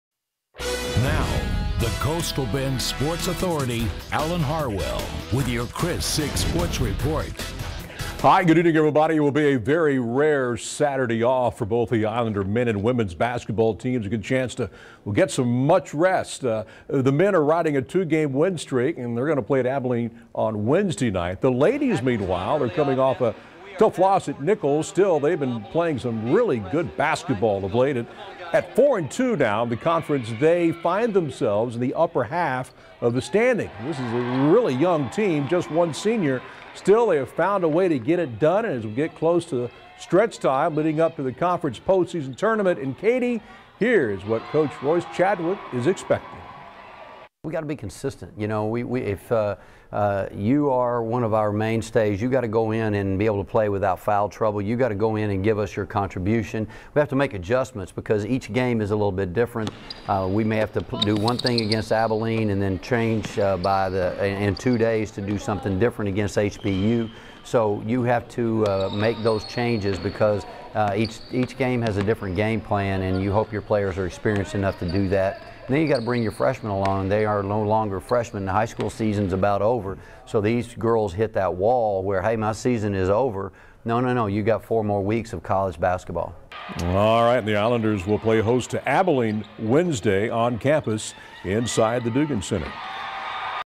Resting Islanders looking for turnaround after struggling loss at Nicholls State
It’s a rare day off for both Texas A&amp;M-Corpus Christi’s men’s and women’s basketball teams. Both will have a chance for some much-needed rest heading into next week.